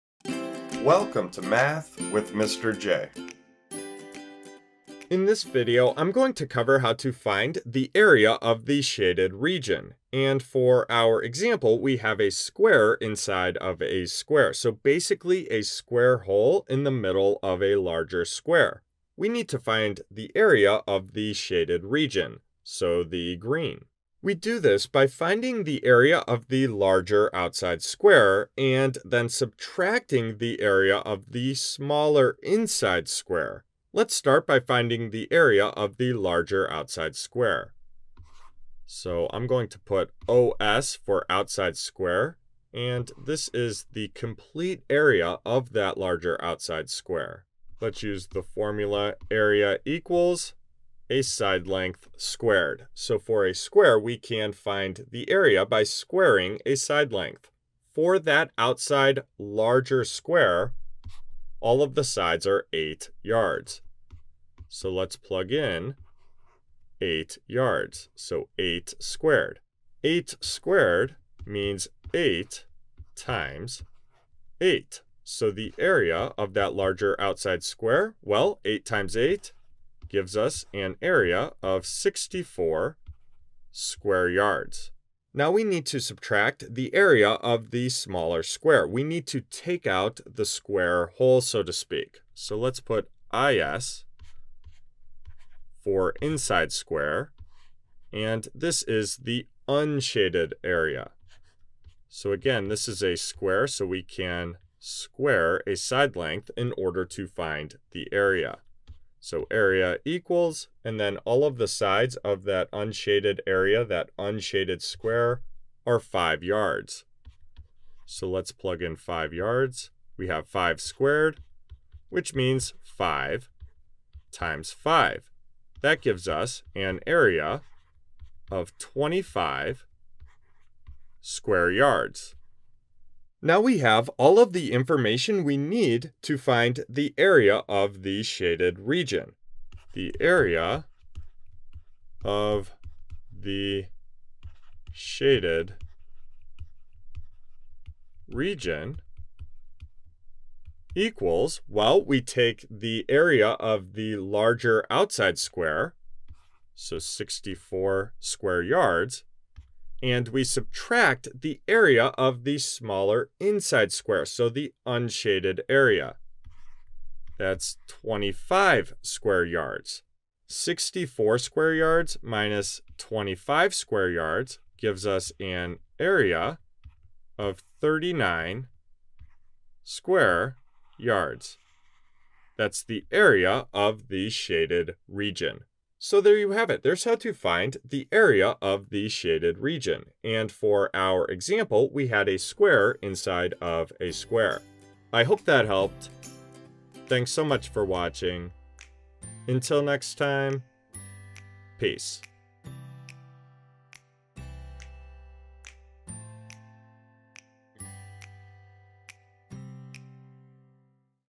How to Find the Area of the Shaded Region | Square in a Square | Math with Mr. J
Welcome to How to Find the Area of the Shaded Region (Square in a Square) with Mr. J! Need help with finding the area of the shaded region? You're in the right place!

Whether you're just starting out, or need a quick refresher, this is the video for you if you're looking for help with how to find the area of a shaded region. Mr. J will go through an example (square in a square) and explain the steps of how to calculate the area of a shaded region.

Hopefully this video is what you're looking for when it comes to finding the area of the shaded region.